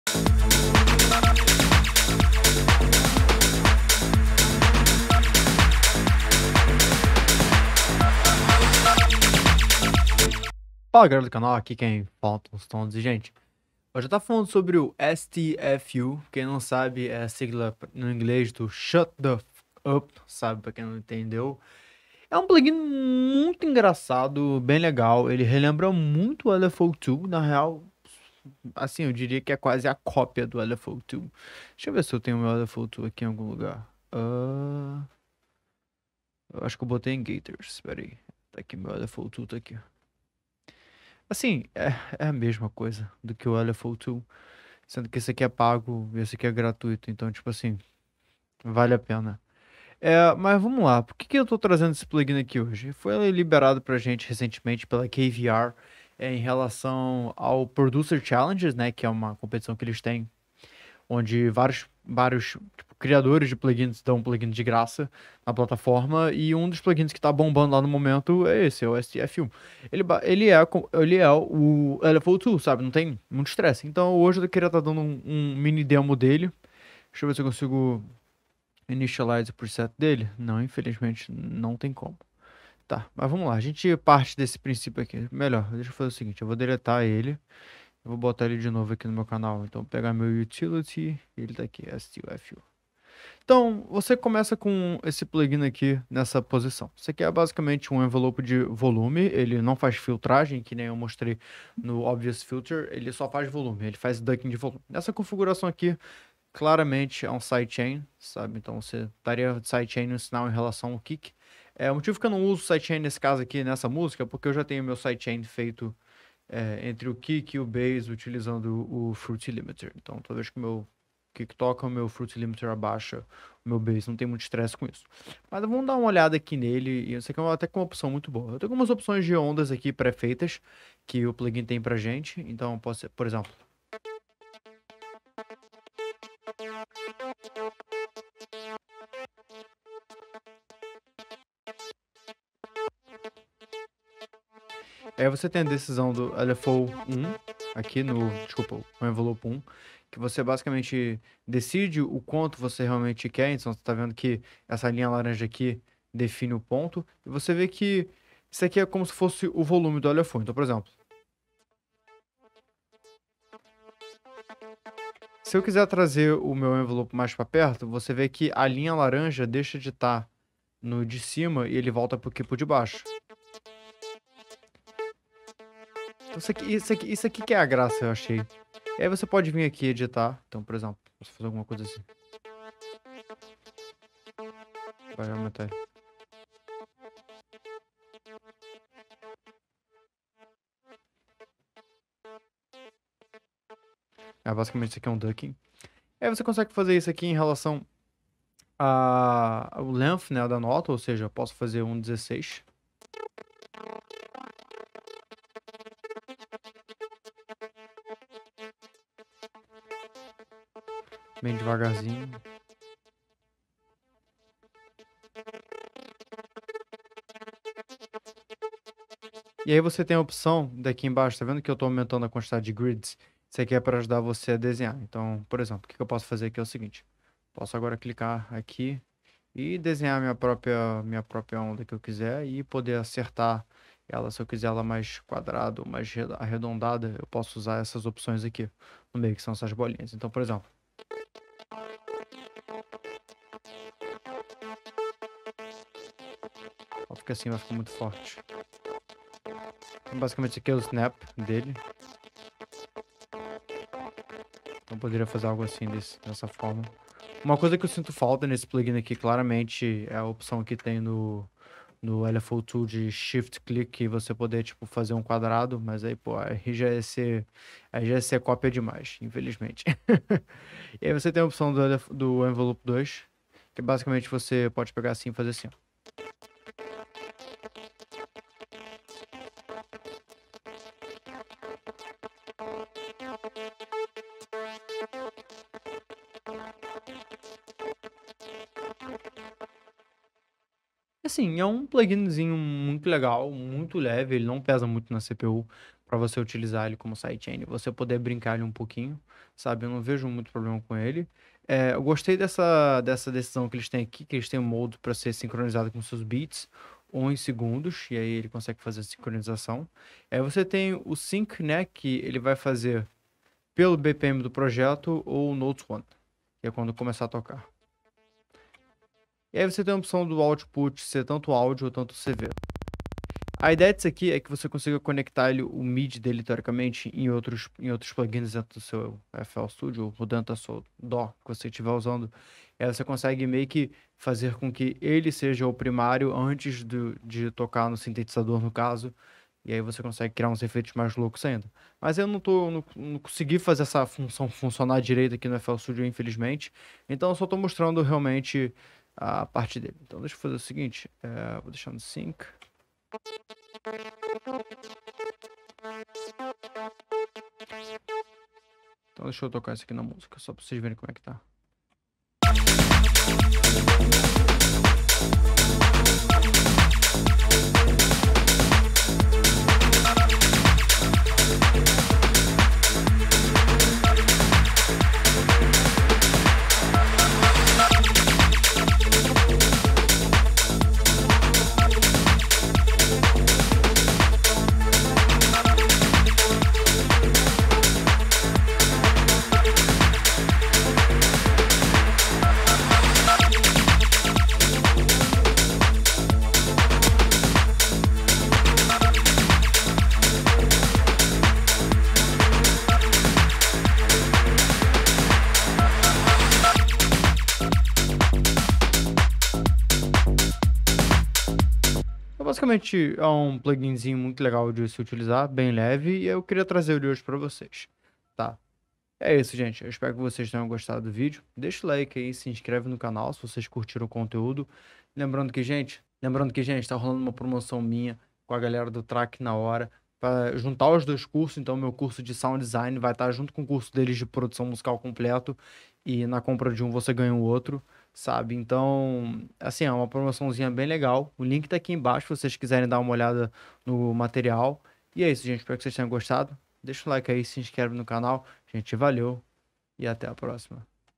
LFO TOOL PERDEU FEIO PARA ESSE PLUGIN GRÁTIS - STFU Volume Shaper
SEGUE Ai NO INSTAGRAM!
VolumeShaper permite moldar os níveis de qualquer sinal de áudio em detalhes precisos. De efeitos de compressão multibanda sidechain a gating extremo, de gagueira sincronizada com o tempo a modelagem precisa de samples - VolumeShaper oferece uma grande variedade de efeitos musicais criativos com uma abordagem visual do volume.

:::::::::Links:::::::::::: (Social)